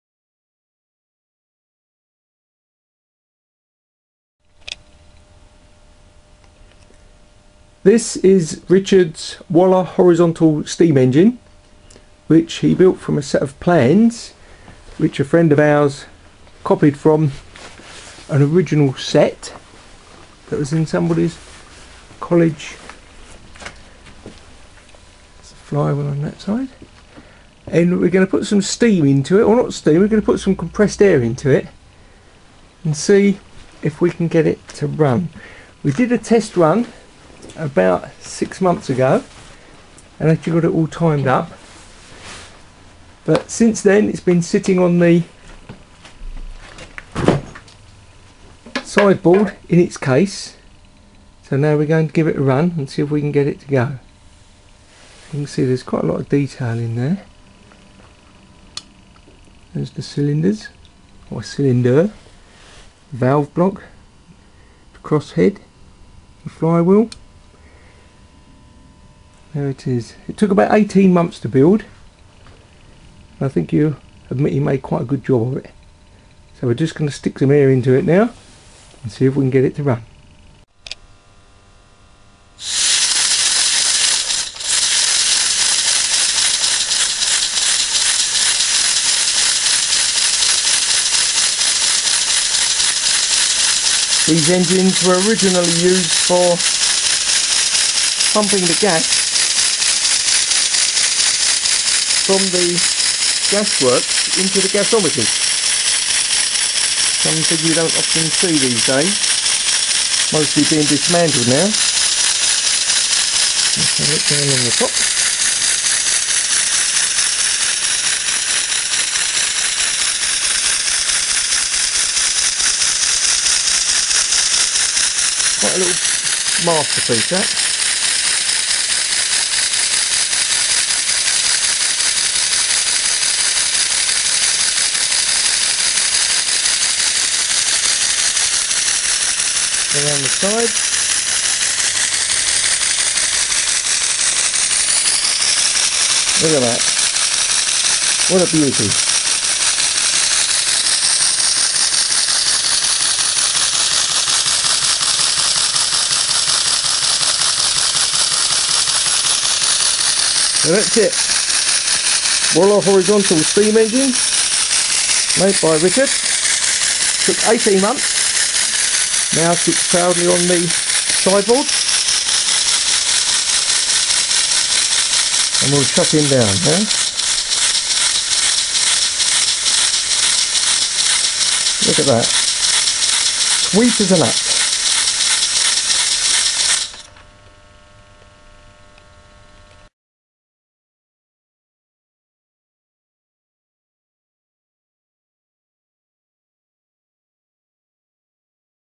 Waller Horizontal Steam Engine
Live steam, Waller horizontal steam engine running on compressed air. 1:12 scale scratch built model.